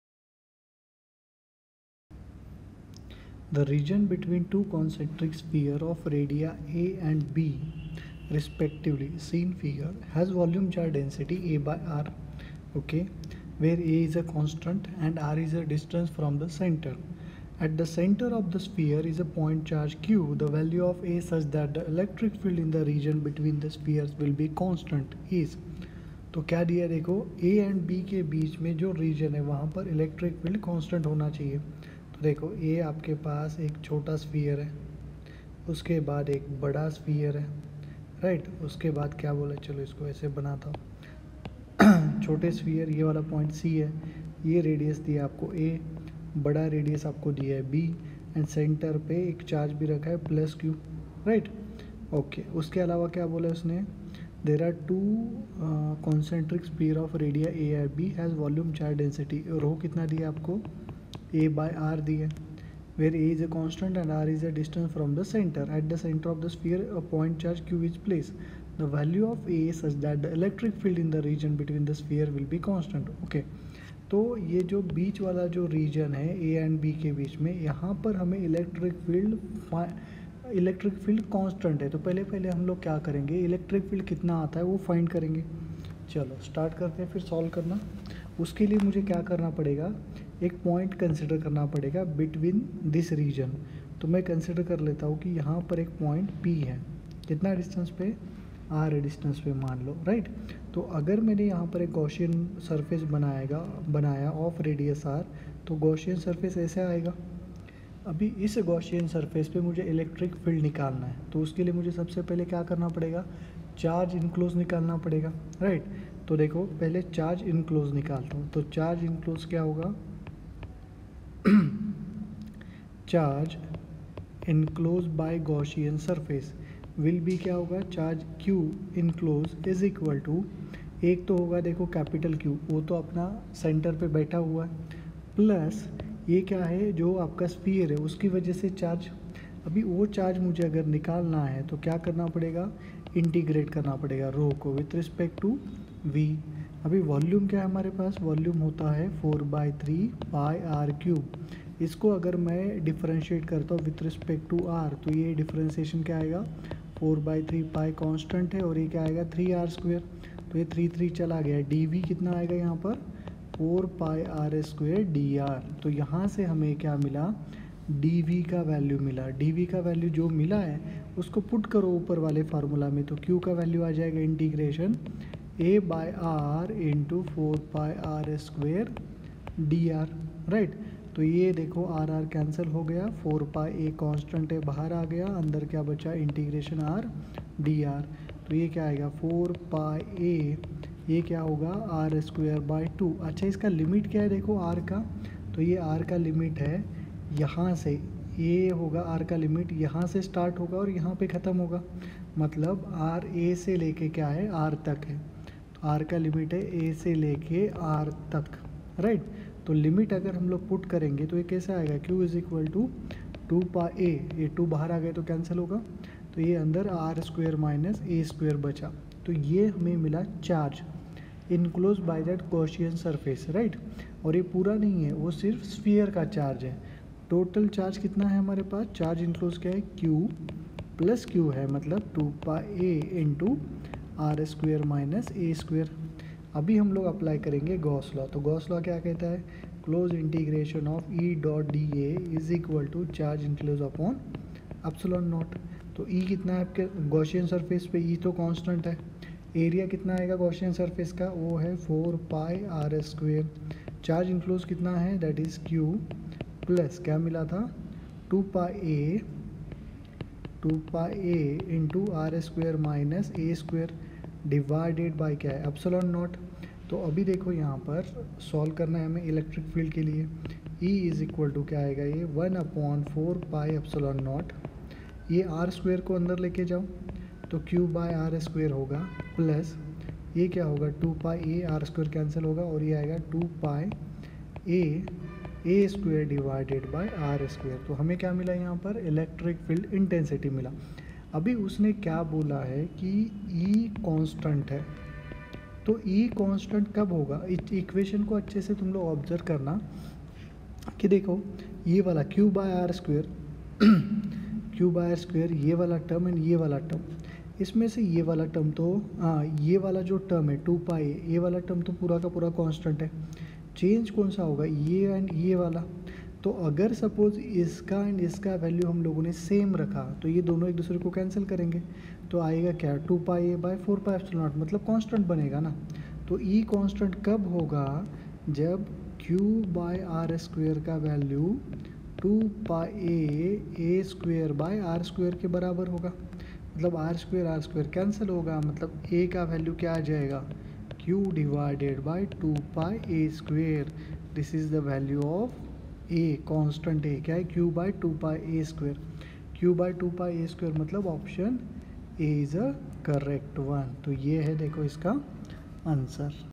The region between two concentric spheres of radii 'a' and 'b', respectively (see figure), has volum
Q 14. The region between two concentric spheres of radii 'a' and 'b', respectively (see figure), has volume charge density =A/r , where A is a constant and r is the distance from the centre. At the centre of the spheres is a point charge Q. The value of A such that the electric field in the region between the spheres will be constant, is

  a. Q/(2πa^2 ) b. Q/2π(b^2-a^2 )

  c. 2Q/π(a^2-b^2 )  d. 2Q/(πa^2 )